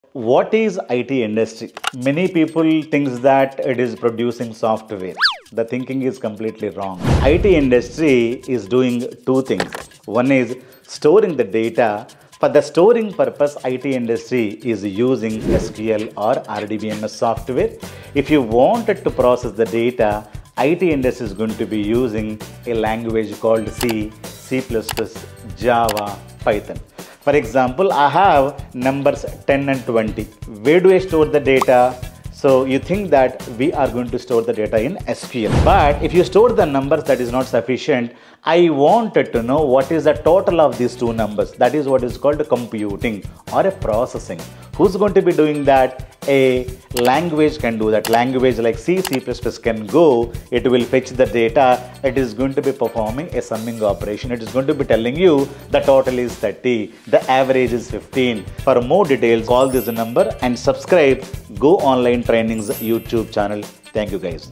🚀 Master These Skills & Stay Ahead in the IT World! 🌍💻 The IT Industry Runs on Two Pillars! 📊
🔹 Storing Data: SQL & RDBMS software keep business data secure and structured.
🔹 Processing Data: Programming languages like C++, Java, and Python bring data to life!

🎯 Why Choose Go Online Trainings?
✅ Learn from industry experts
✅ Hands-on real-time projects
✅ Master Data Analytics, Data Science (AI), Data Engineering, DevOps, and Cloud Technologies (Azure & AWS)

💡 Your IT Career Starts Here!